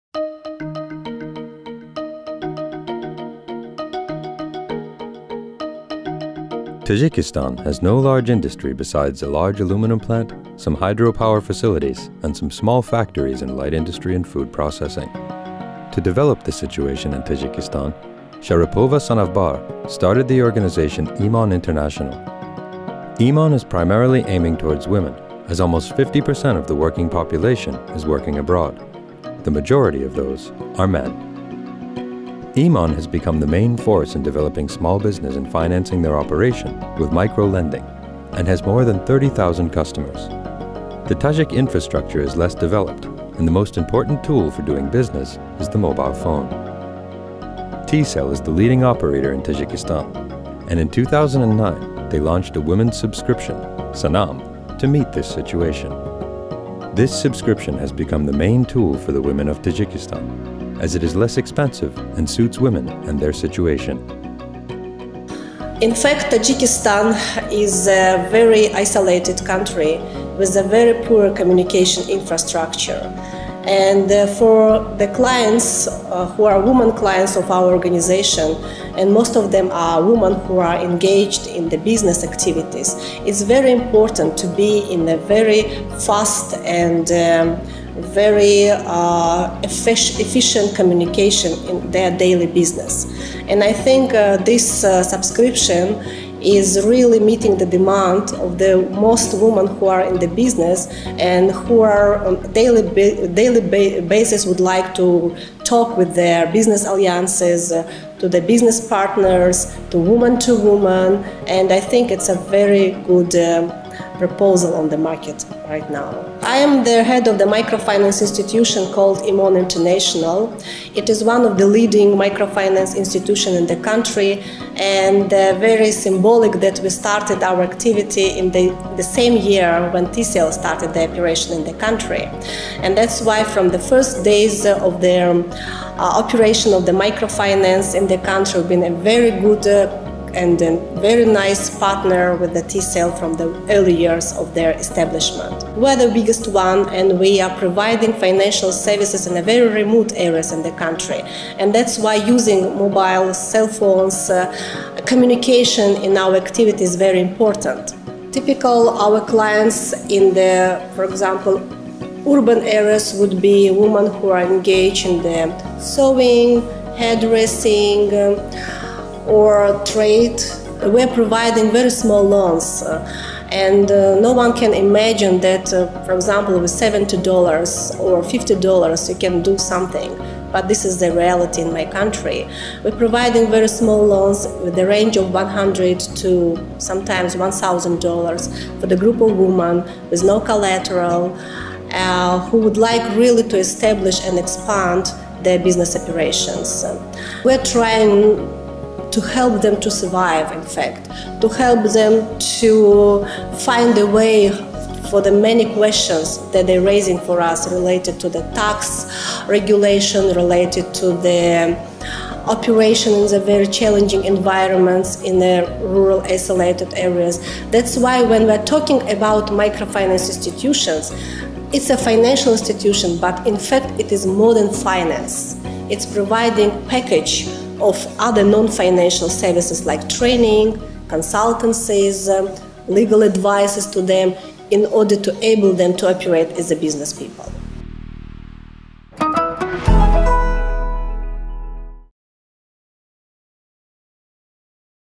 Mobile subscription helps empower women in Tajikistan
While two-thirds of Tajikistan's population lives in poverty, the lack of employment opportunities has led to almost half of the working population of Tajikistan to migrate abroad for work. Since it is mostly men who leave the country to find jobs abroad, Tajikistan's population is dominated by women. In 2009, Tcell launched a special tariff plan for women in South of Tajikistan, to empower and enable women to stay better connected with family and friends, improve their safety, and help them obtain paid work.